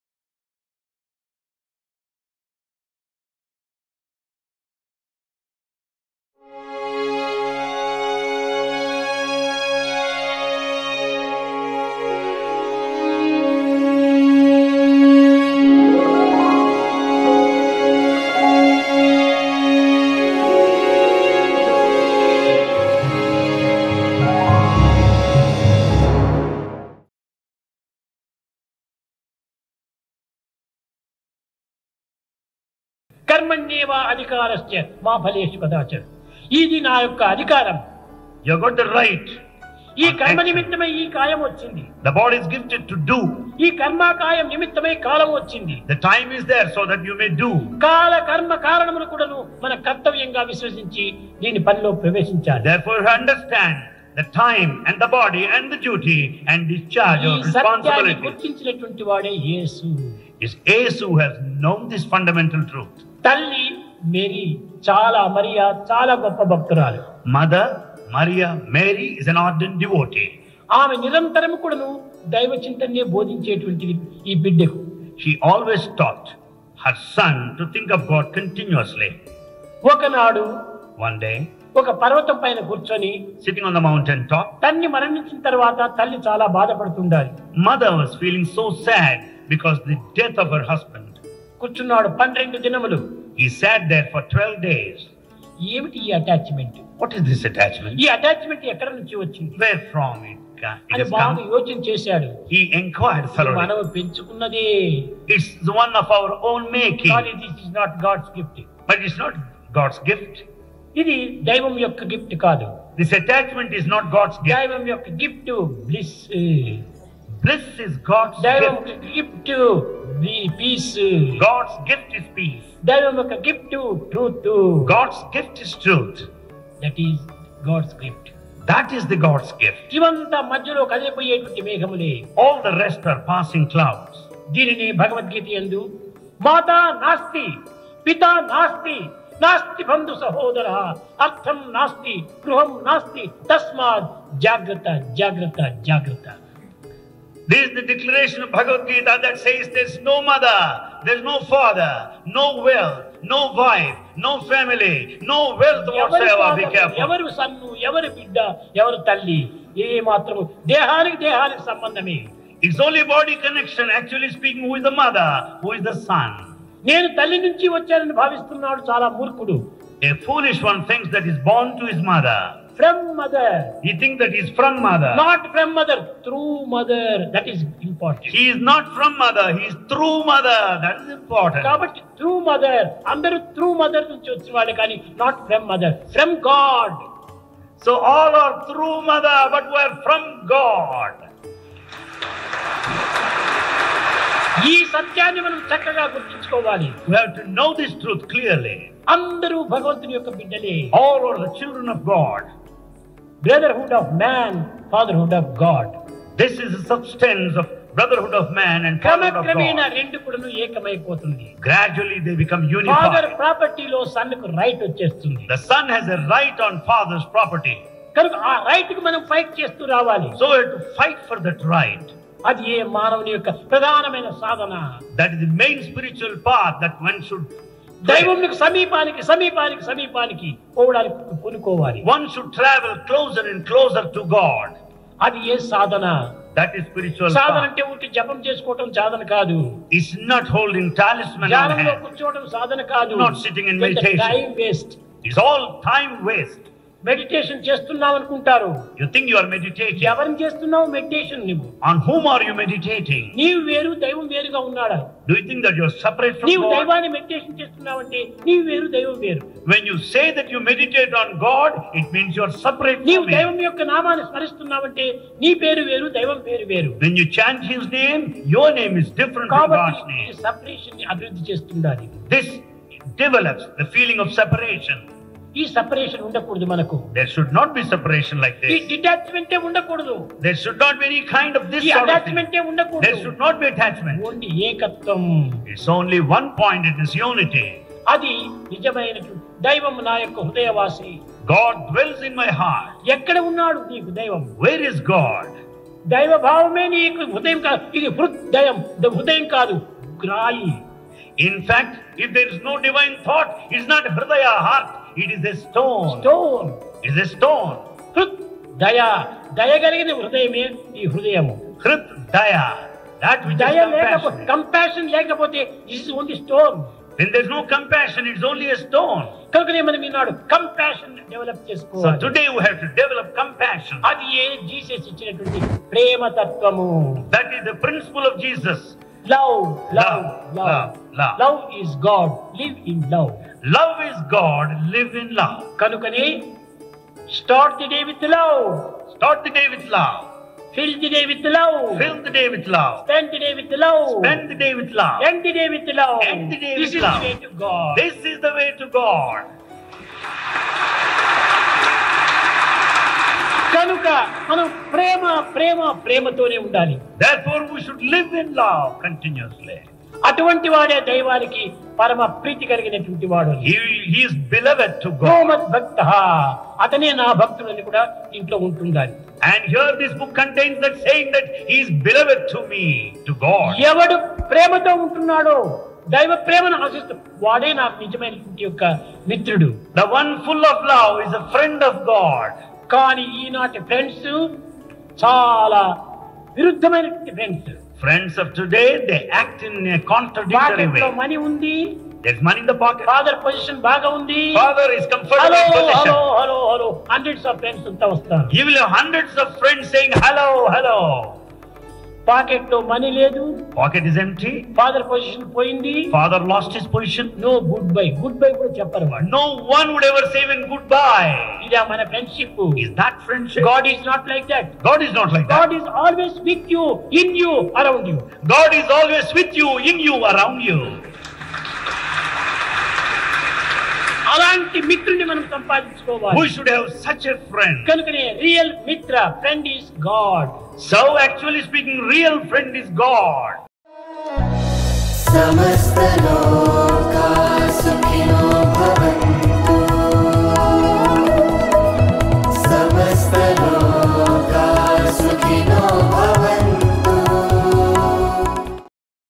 What is the Message of Jesus on Relationship with God? | Sri Sathya Sai Answers
Over a billion people across the globe will celebrate the birth of Jesus Christ this week. While Jesus is revered as the 'Son of God', what relationship should we be cultivating with God?

In this video, Sri Sathya Sai Answers this question.

Contents of this Video:
00:32 - How should we conduct ourselves?
00:57 - What revelation did Jesus receive on his father's death?
02:35 - Where have we come from?
03:47 - What Relationship should we build with God?
05:19 - What is the main Principle of Jesus?